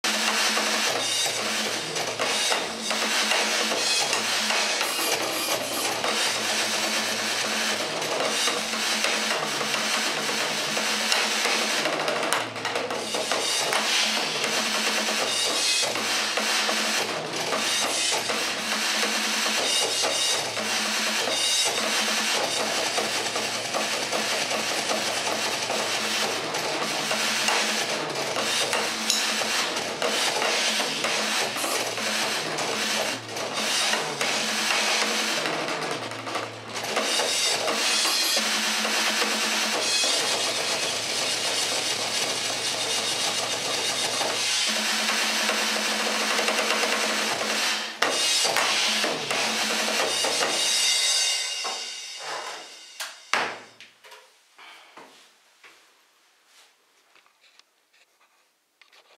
Roland TD-7 (Stefan / Die Knappen)
Roland TD-7 von 1995, privat beim Drummer der Knappen